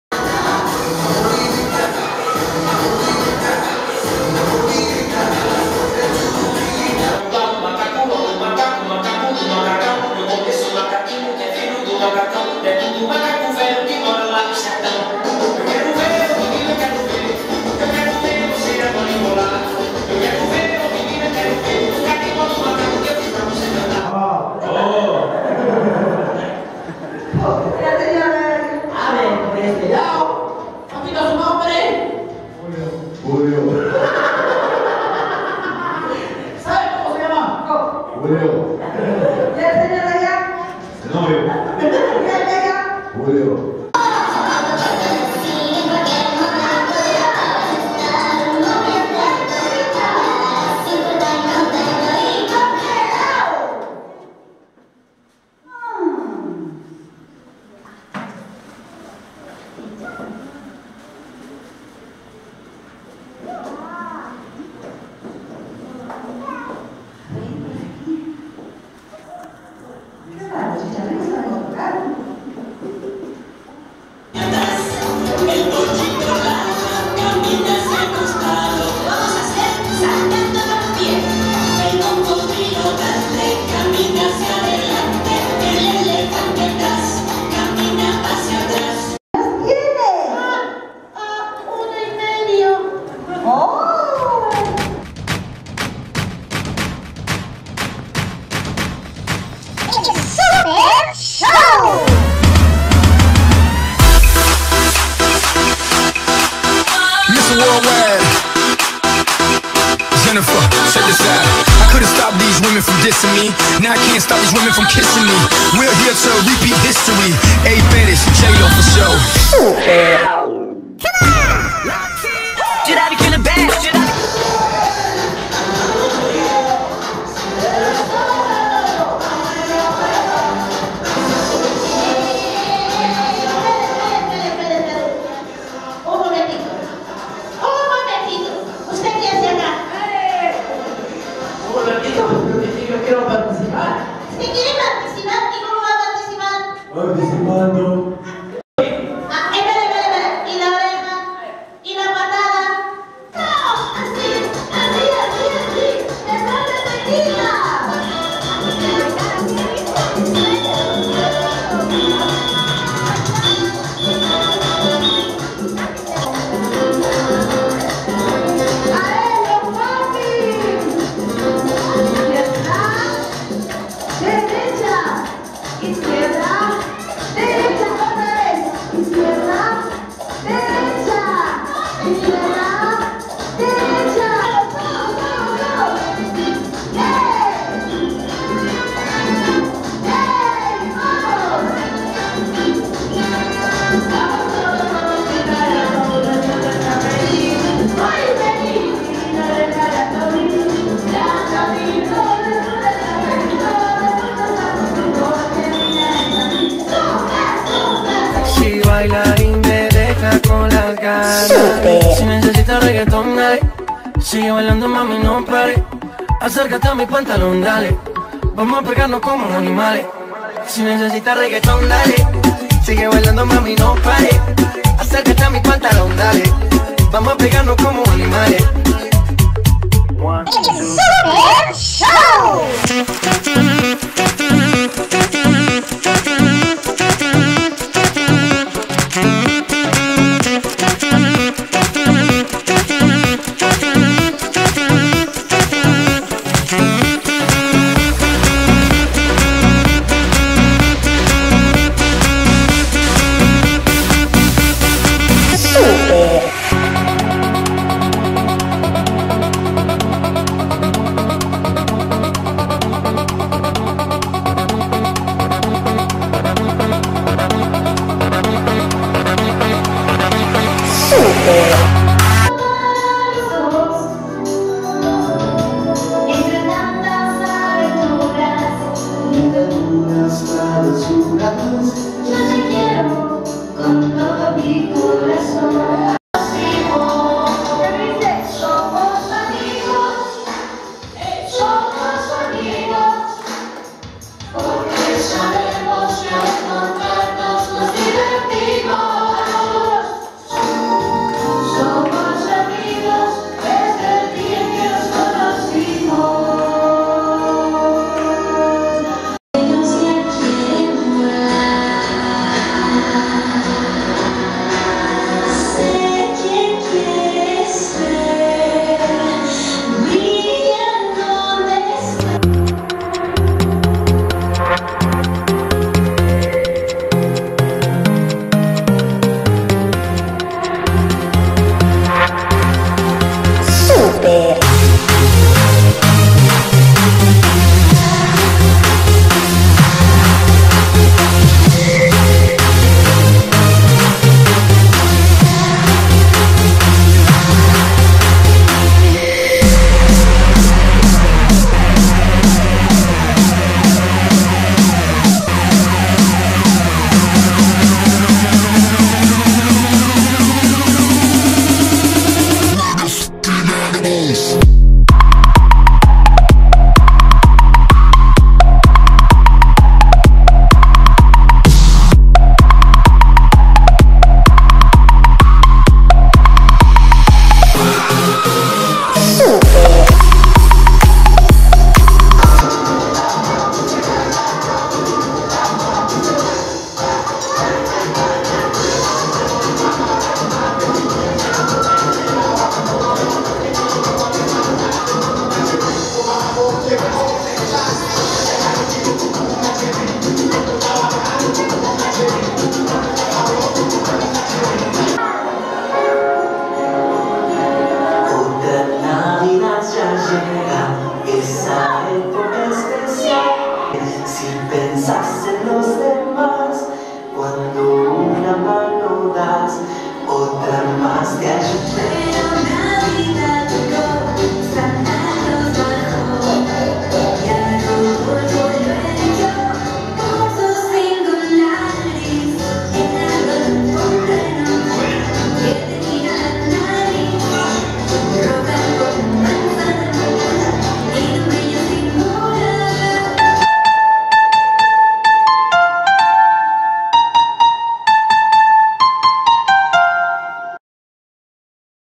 EL SUPER SHOW, LLEVO UNA BELLA FIESTA
CONTINUANDO CON LAS CELEBRACIONES POR NAVIDAD, ALINA PEKITA CHICHARRIN Y CHASCOSITO LLEVARON UNA SUPER FIESTA